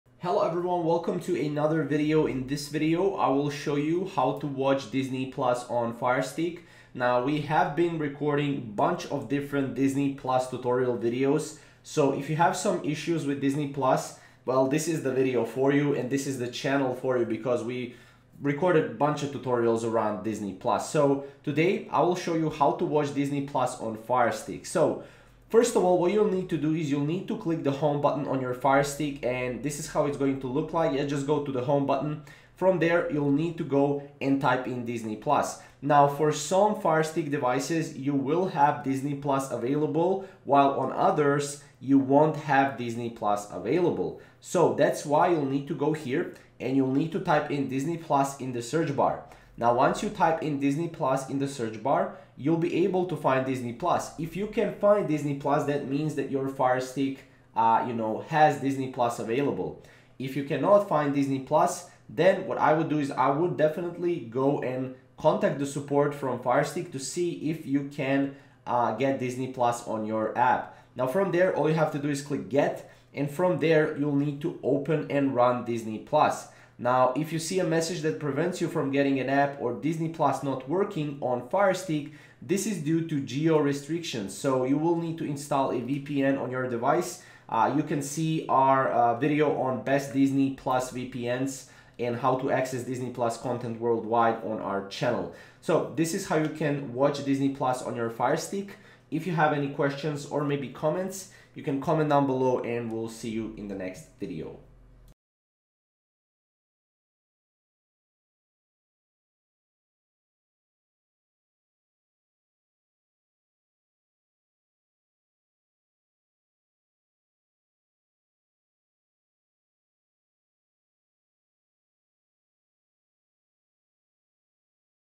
How to Watch Disney Plus on Firestick (Tutorial)
In this video, I will show you how to watch disney plus on firestick